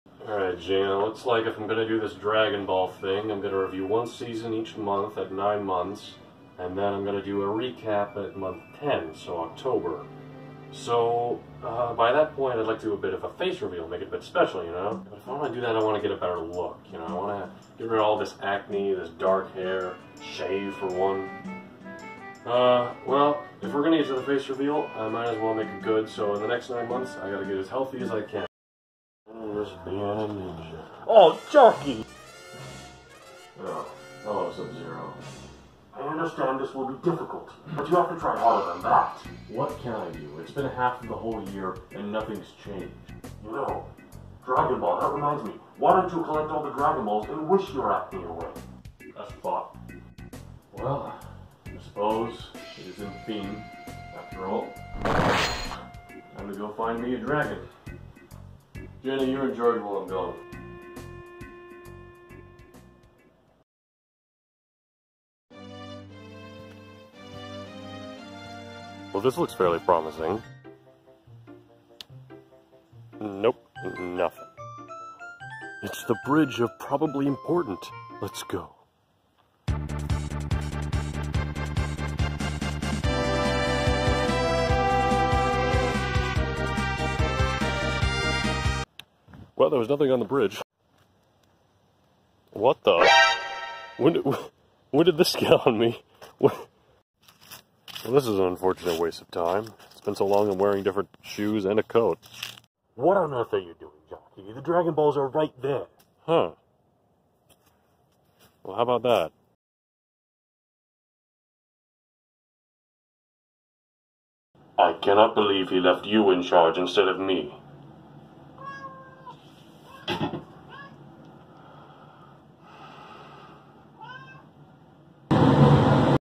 Irrelevant, Ignored, Inconsistent, and Insignificant Sketch Video
Salutations all, and here we have a sketch video I had planned to release with my Dragon Ball Recap. As I went on I ultimately decided not to use it but here it is on it's own anyway just so you all can see for yourselves! I am finally back for the year by the way so regular uploading will begin tomorrow!